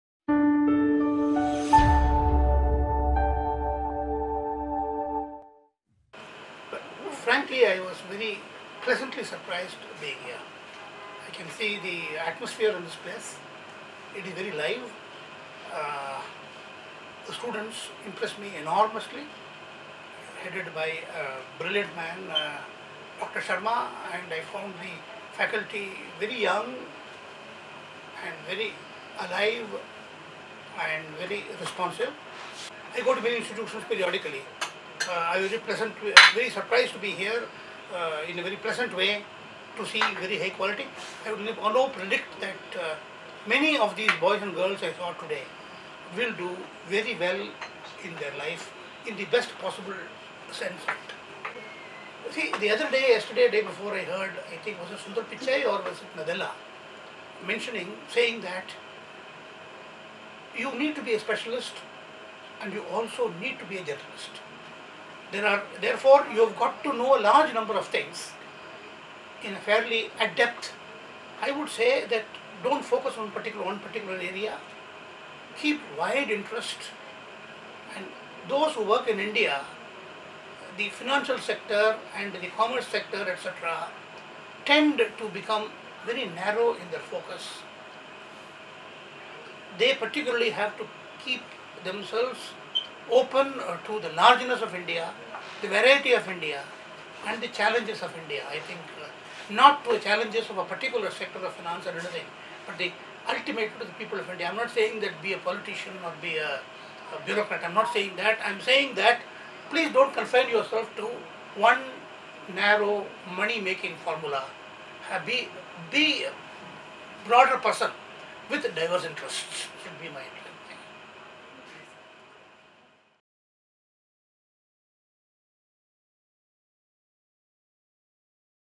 Mr. TSR Subramanian delivered a talk at IBS Gurgaon
Mr. TSR Subramanian, former Cabinet Secretary, GOI, and Chairman  of New  National Education Policy of 2016 delivered a talk at IBS Gurgaon